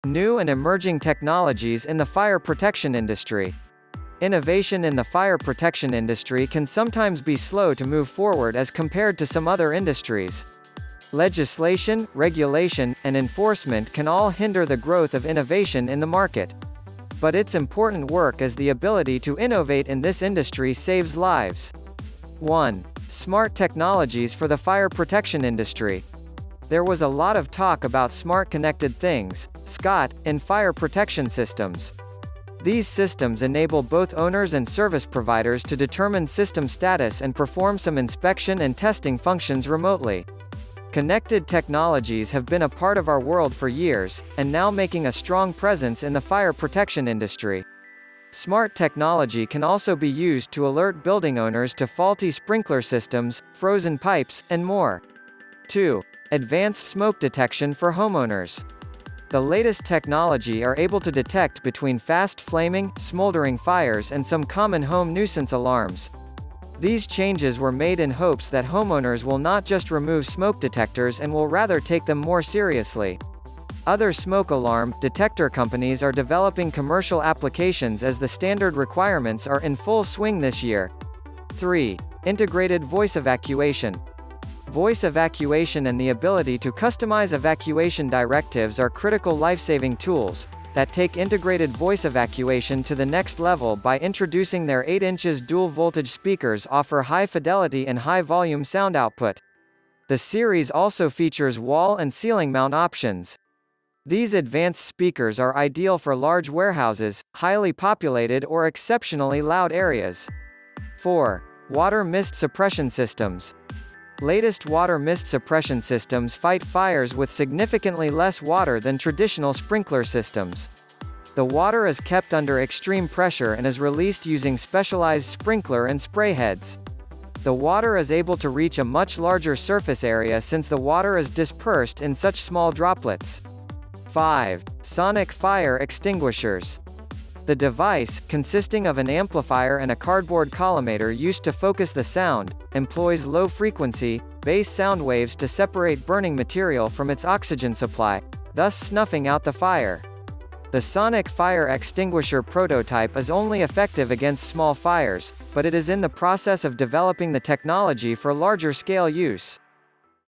New and Emerging Technologies in the Fire Protection Industry
The growing complexities of today’s infrastructure and the demand for more sustainable solutions also fuels the need for innovation. Thankfully there are many companies at the forefront of developing solutions for better, safer, and more efficient ways to prevent and extinguish fires.